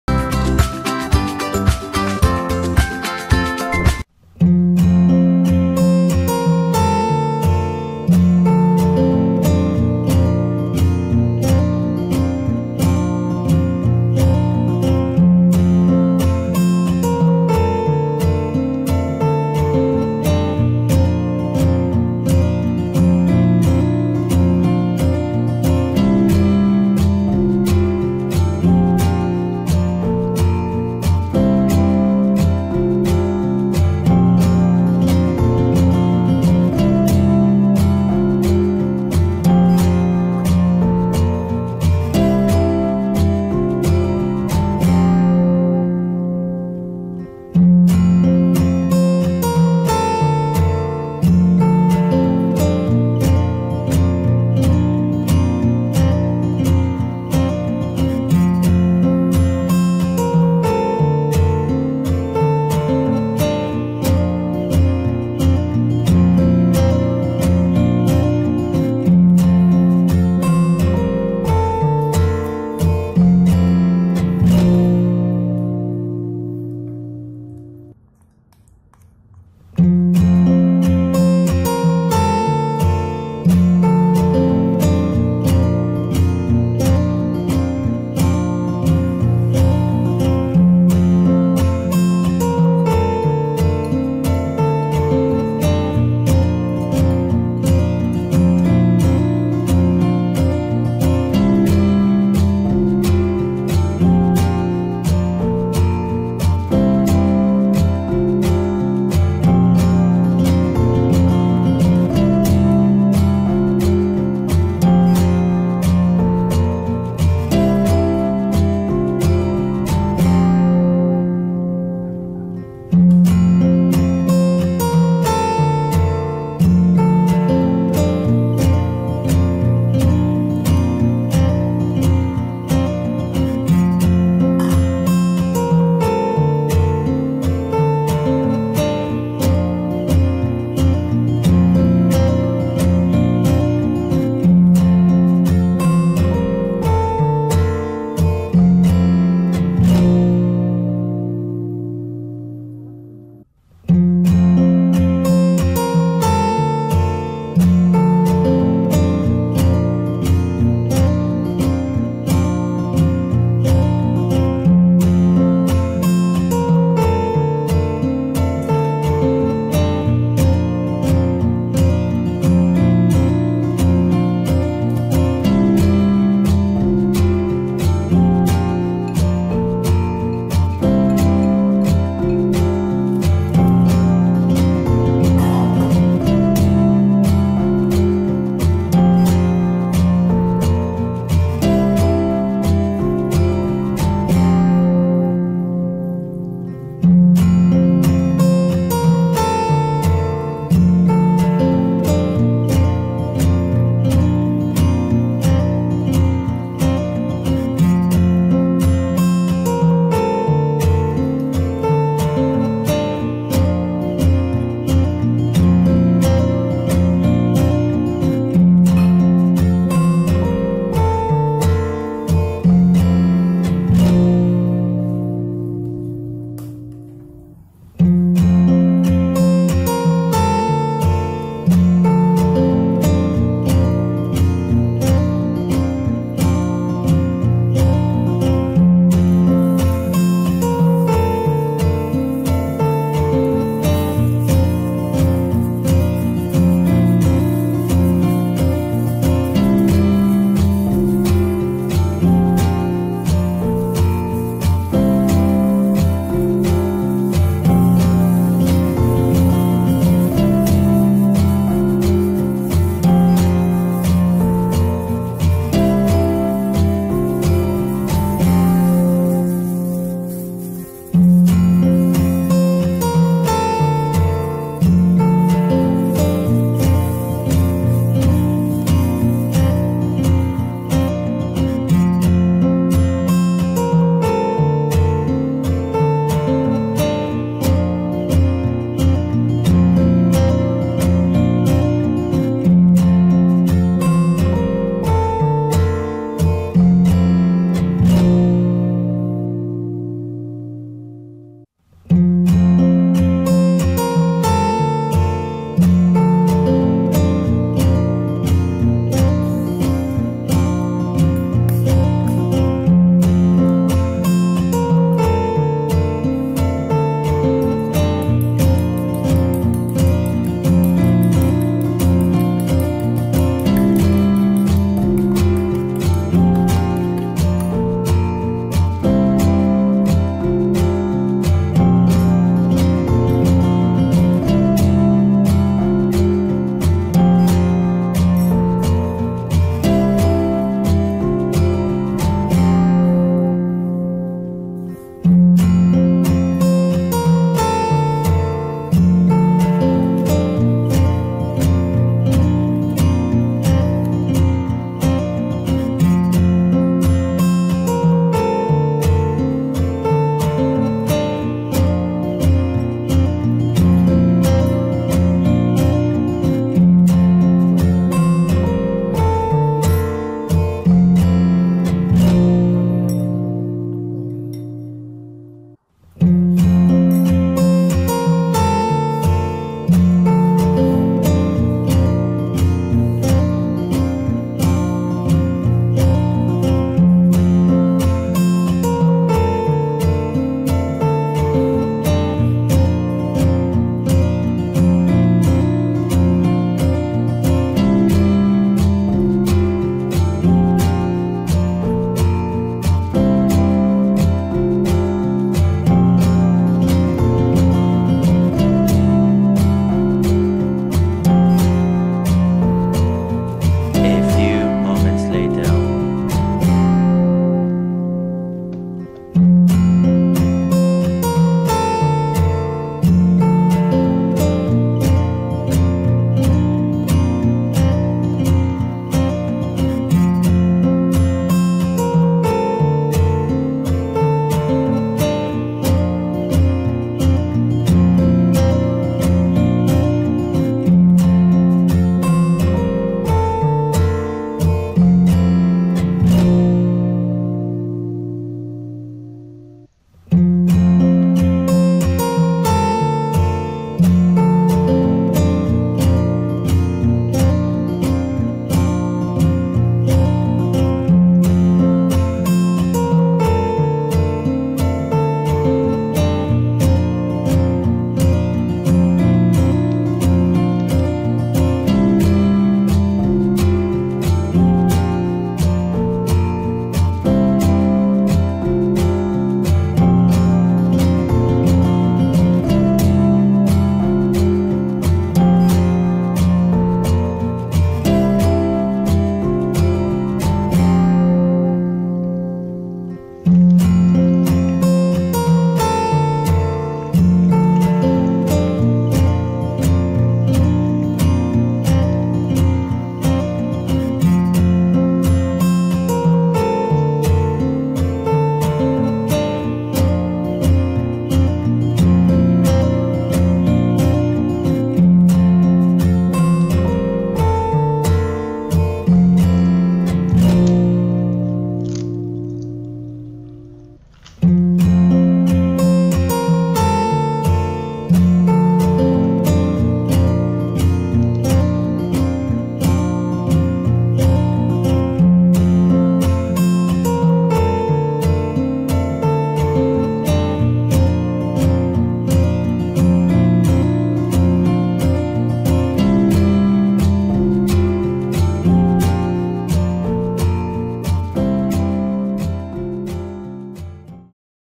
Brulee bomb ala Nagita Slavina Jajanan Kekinian, mudah dan Enak
Hai sedulur semua, kali ini Pawon ceria mau recook jajanan artis ala nagita slavina. Jajanan ini namanya Brulee bomb, belakangan ini sangat booming hingga banyak yang merecooknya. bahannya sangat mudah didapatkan dan bagi yang suka keju, jajanan ini sangat recommended banget.

RESEP :

Bahan
- 6 sdm  Tepung Terigu ( 60 gram )
- 250 ml Susu Cair
- 50 gram Margarin
- 3 bh Sosis
- 50 gram Keju Melted / Keju Cheddar
- Tepung Roti secukupnya
- Minyak untuk mengoreng
- Keju Mozzarella untuk isian

Bumbu
- 1/2 Bawang Bombay
- 1 Siung Bawang Putih
- 1/2 sdt Garam
- 1 sdt Penyedap rasa
- 1/2 sdt Merica

TIPS :
- Minyak harus sudah panas
- Gunakan api sedang untuk mengoreng
- Jangan mengoreng terlalu lama

Follow IG : @aderacahya

Terima kasih sudah mampir dan menonton
Pawon Ceria akan selalu meningkatkan kualitas Content, Kritik dan Saran Comment dibawah yaaa